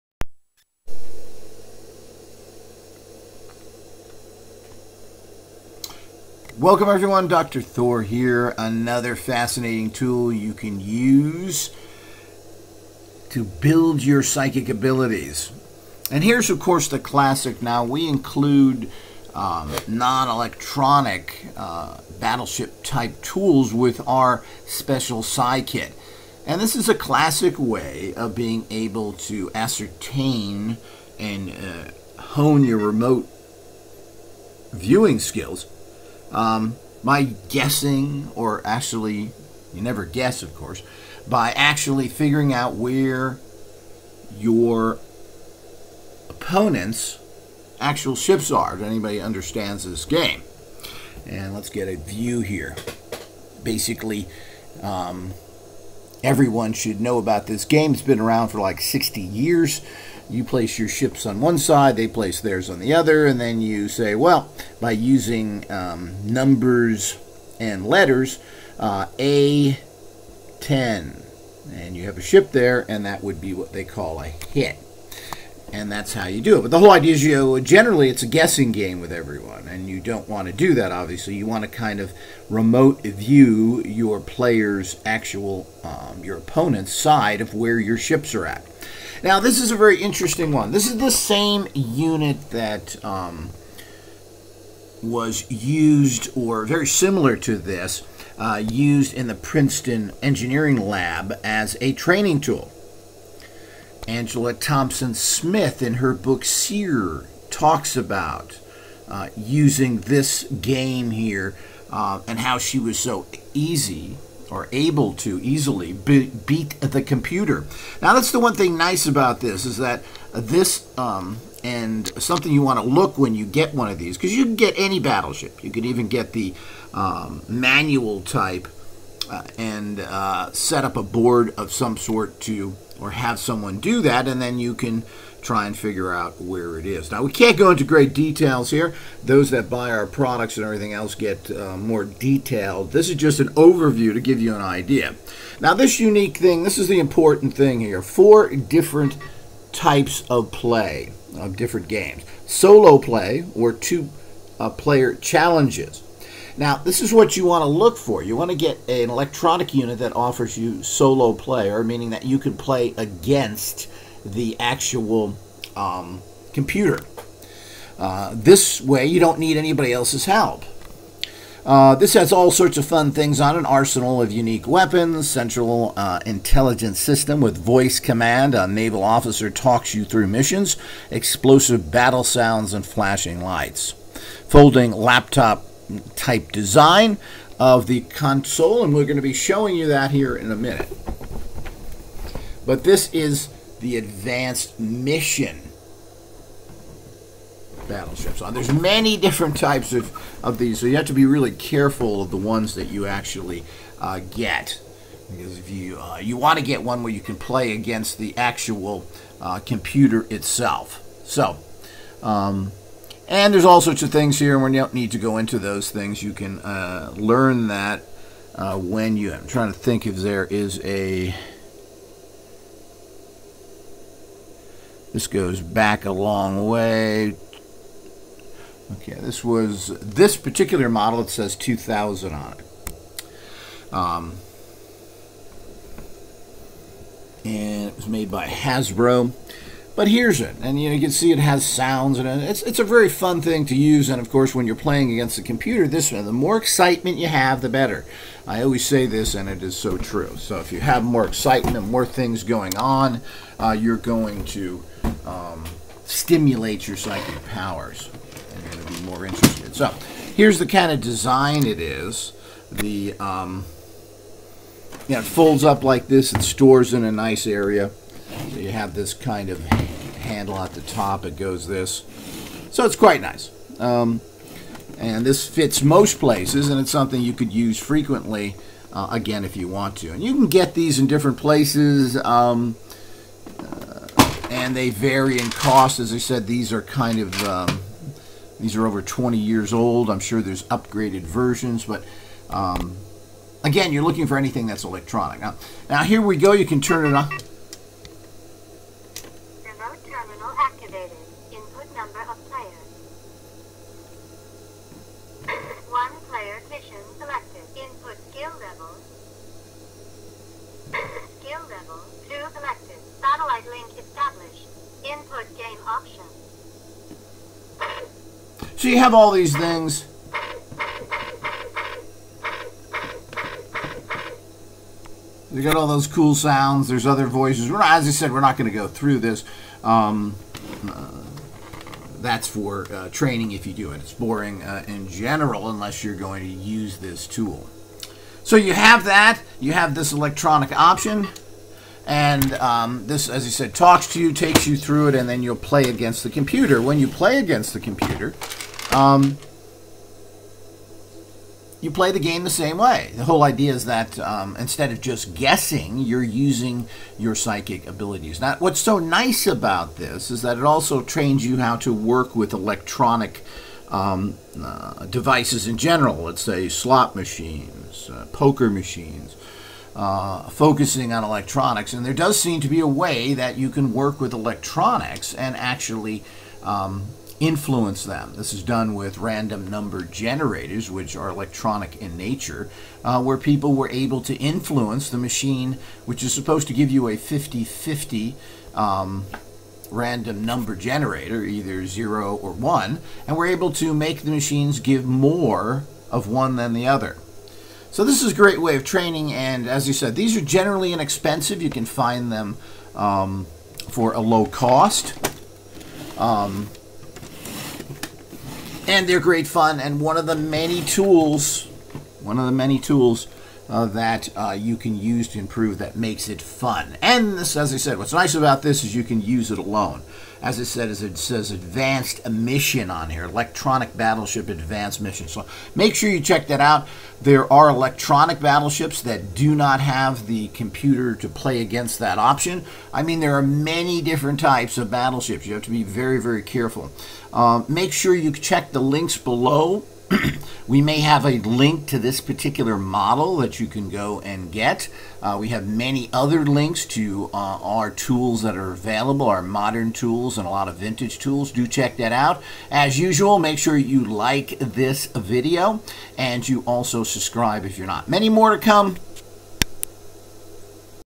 Battleship Advanced Electronic Edition Board Game - Psychically Beat The Computer!
This tool was used by Princeton Engineering & Angela Thompson Smith to develop their remote viewing and psychic skills. SEE IT IN ACTION!